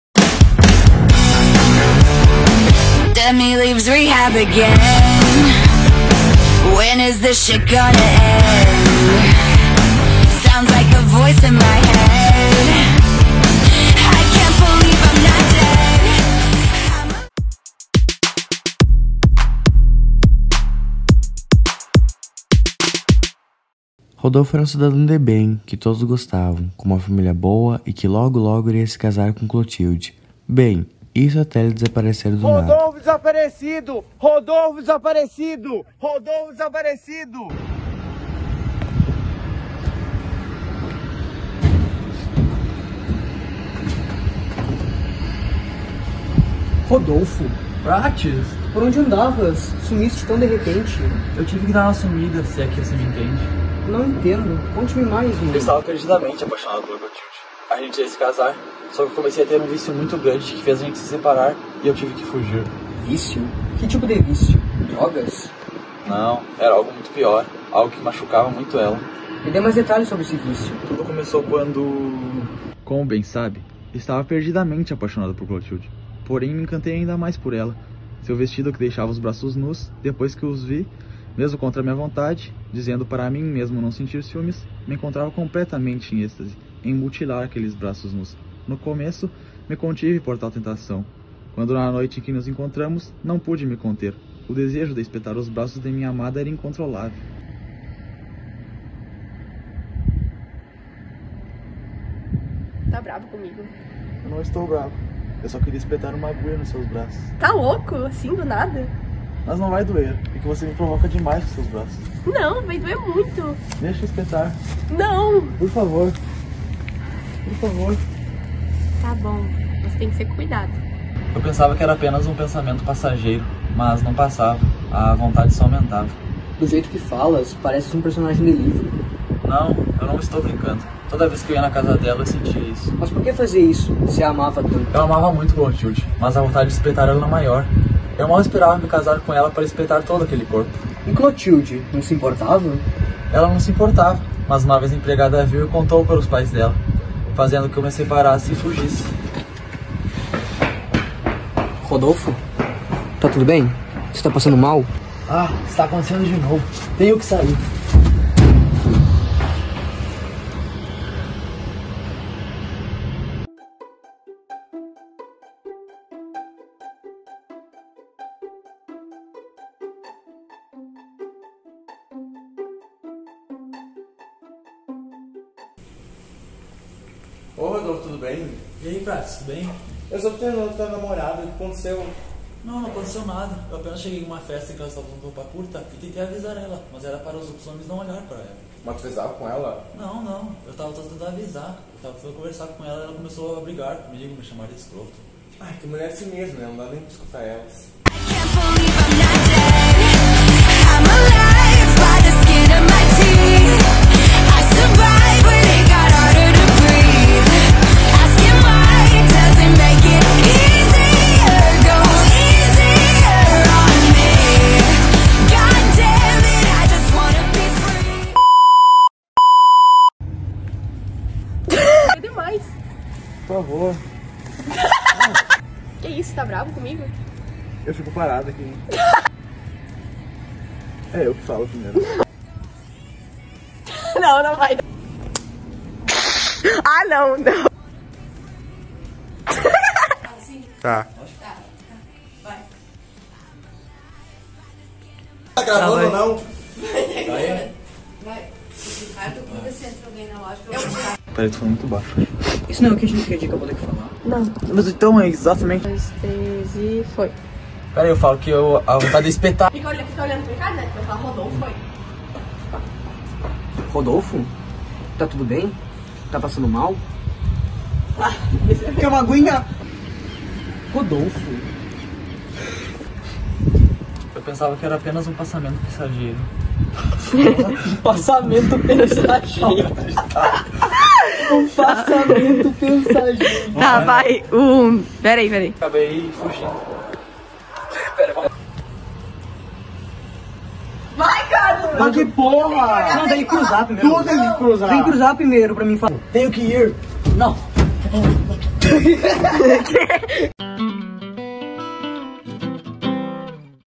Curta-metragem - Dentro da Noite (Conto João do Rio)
Curta-metragem - Conto: Dentro da Noite
Autor: João do Rio

Atores:
Rodolffo - Ricardo Monego
Clotilde - Carolina Pedron
Justino (Prates) - Carlos Adriano (1° fase)
 / João Prates (2° fase)
Figuração - João Prates
Narração: João Prates
Edição: João Prates
Roteirização: Carolina Pedron
3° MSIA

Intro 1: Dolby Atmos HD (adaptado)
Intro 2: Dark Castle Movies (adaptado)
Câmera: iPhone 11 / Nokia 5.4 (12mpx)
Gravações: Cacequi - RS